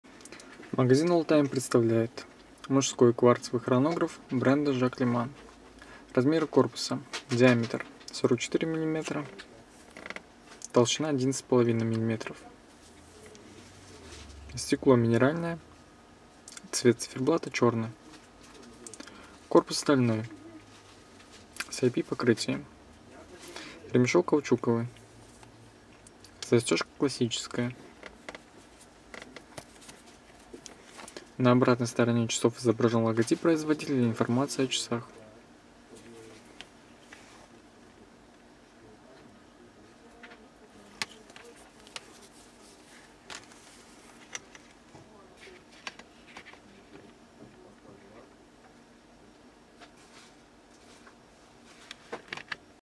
Обзор наручных часов Jacques Lemans 1-1799E с хронографом
Часы Jacques Lemans для тех, кто стремится к высочайшему качеству, но при этом не считает нужным переплачивать за раскрученный бренд или супермодный дизайн. Компания всегда придерживалась принципа: «Высокому качеству — разумная цена». Предлагаем к просмотру видеообзор мужских наручных часов с хронографом, который смотрится особенно очаровательно, благодаря яркой секундной стрелке - Jacques Lemans 1-1799E